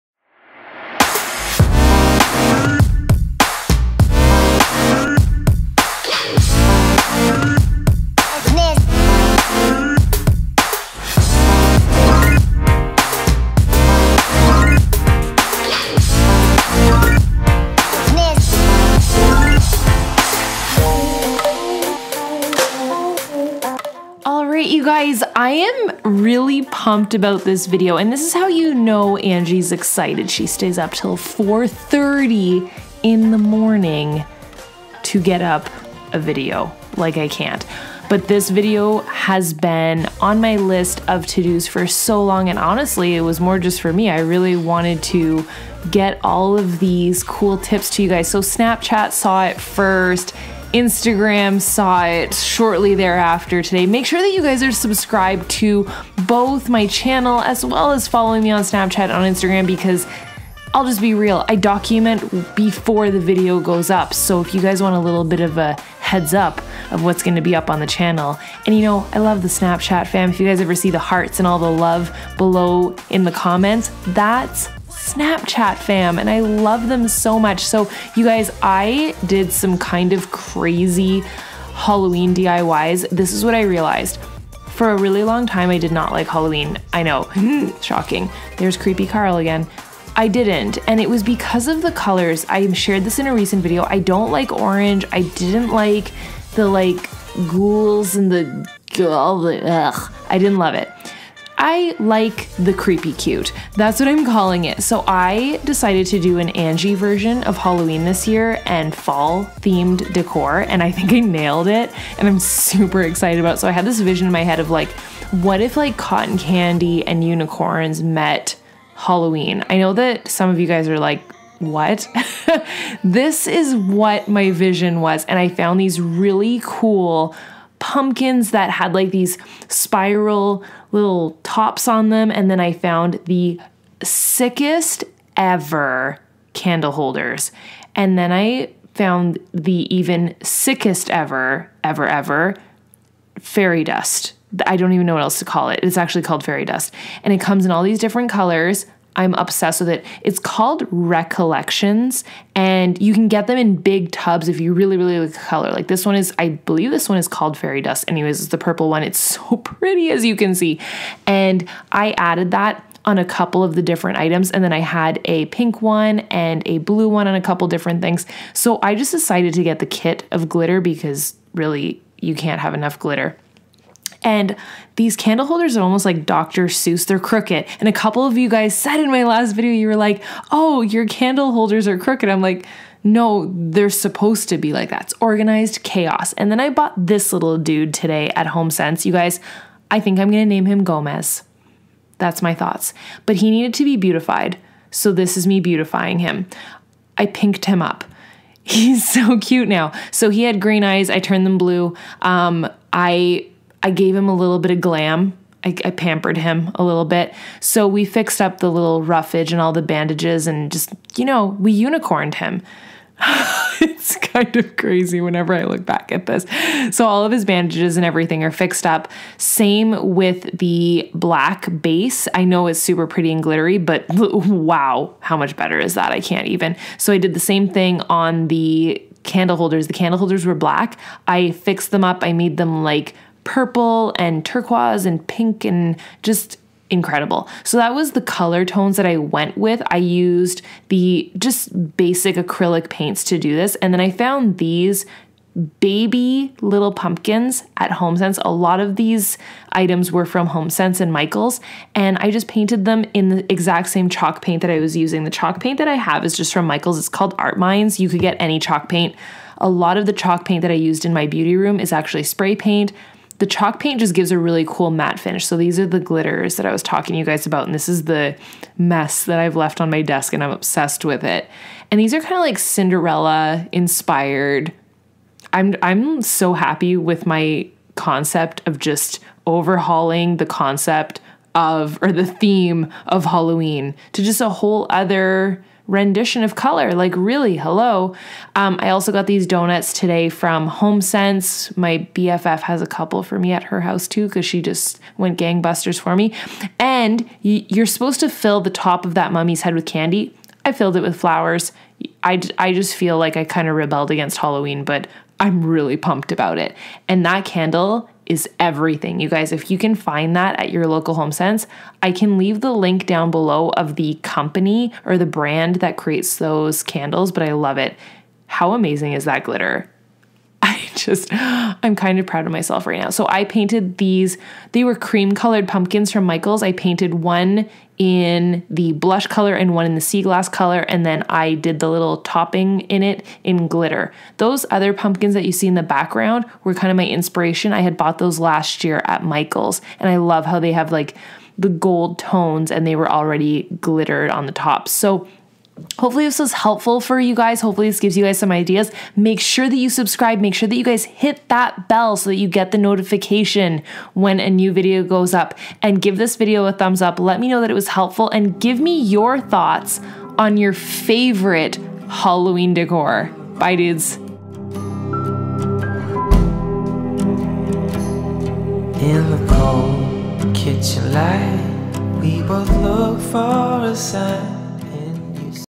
Creepy Cute Pastel Decor - Fall & Halloween DIY ideas
Halloween and Fall DIY ideas - Creepy Cute Pastel Decor

Pumpkins - Michaels & Homesense
Chalk Paint - Michaels
Candle holders - Michaels
Mummy - Homesense